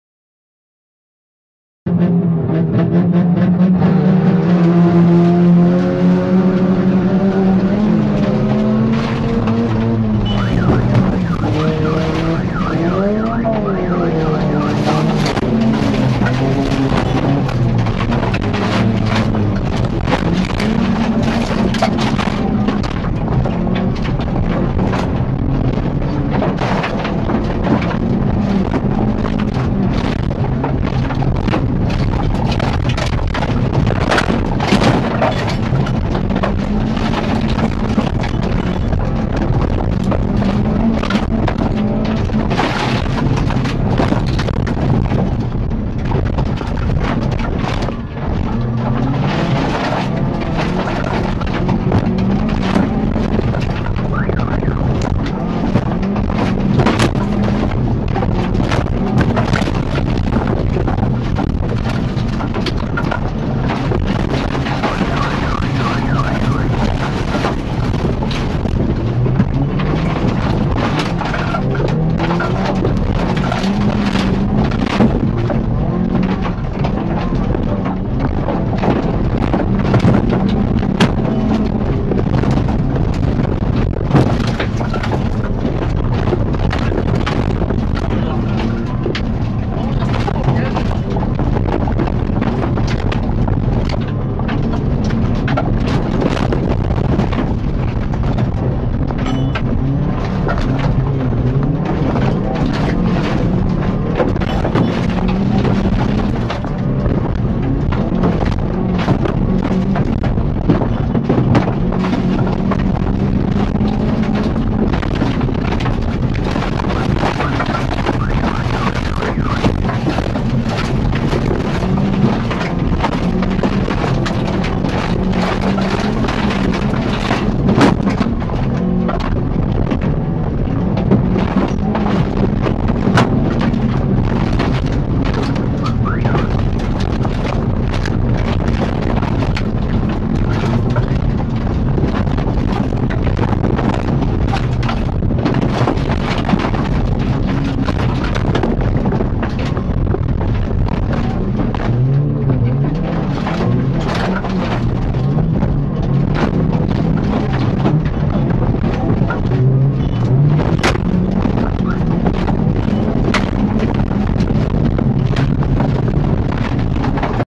Wrak Race Silesia Gliwice 25.08.19r. Wnuk/Starosta Onboard
Film ze środka Wrak race silesia, samochód Mitsubishi Colt.
Film z 6 dywizji biegu ćwierfinałowego zakończony na 1 miejscu, niestety uciety przez kamere.